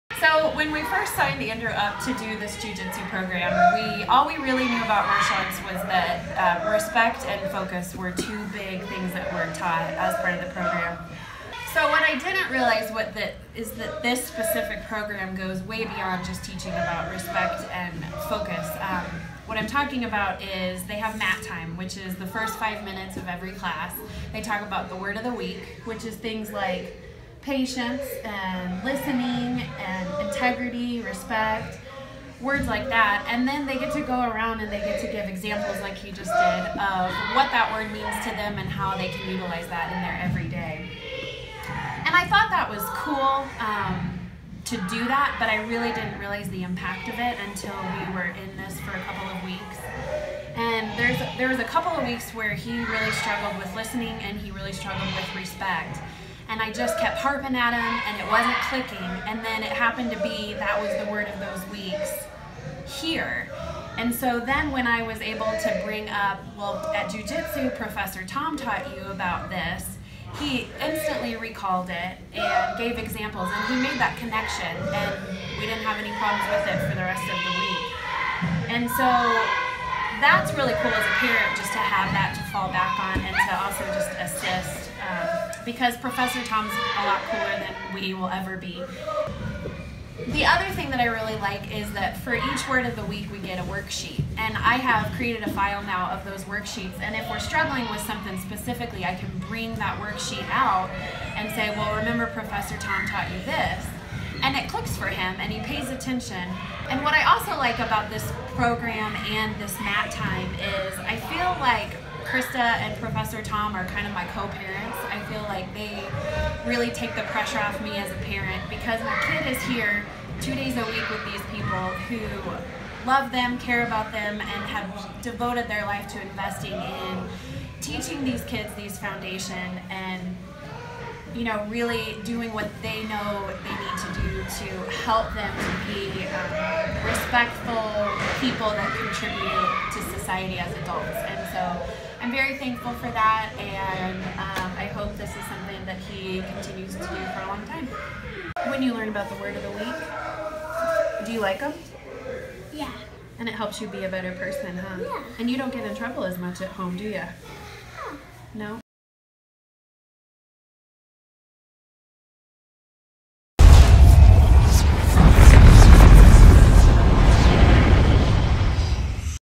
Testimonial about our word of the week for Kids Martial Arts program in Monument
Testimonial from Andrew's mom talking about the Word of the Week in the Kids Martial Arts classes.

Instagram.com/monumentjiujitsu
Facebook.com/monumentjiujitsu